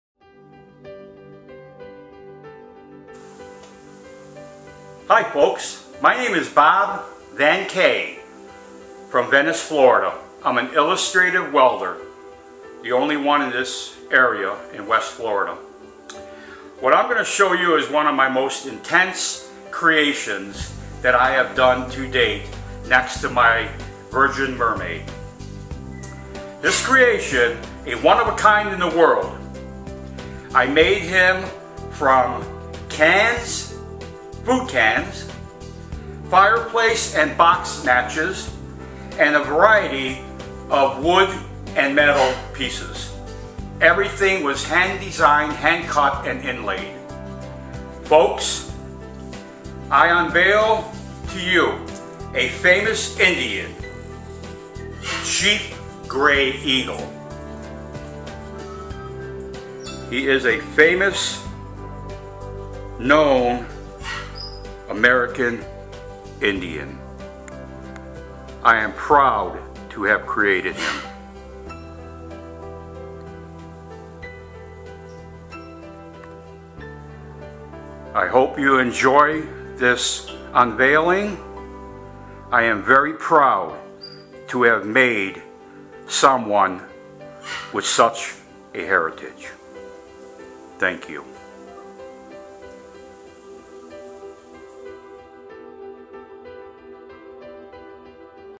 Bob Van Kay, Venice, FL sculptures a Famous American
The American Indians, as far as I am concerned and many others, are the "True Americans" of our country. I give them the greatest respect. That is why I am so proud to have made a sculpture of Chief Grey Eagle. Besides him, there are many other famous Indians and everyone should give honor to them.